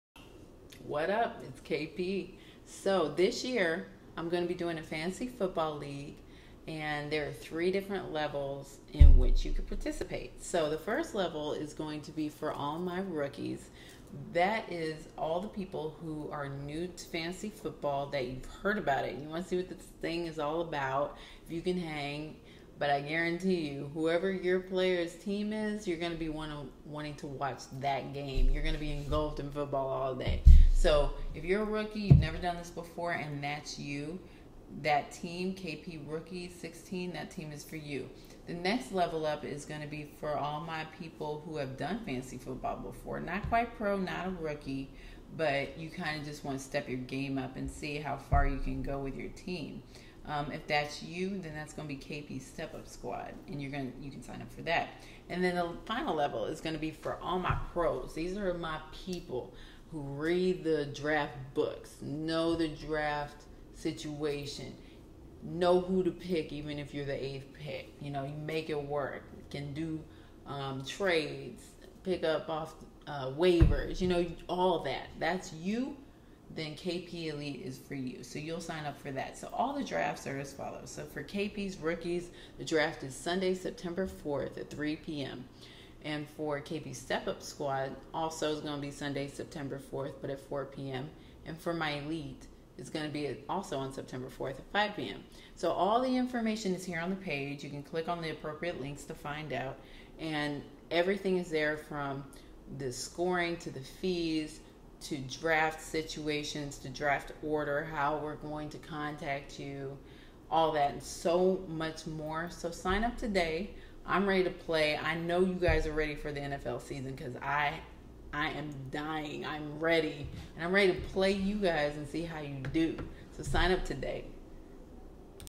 Sign Up for Fantasy Football!
Another year, another season of Fantasy Football! Sign up whether it's your first time or you're a pro! Say goodbye to your family for every Sunday until February 2017!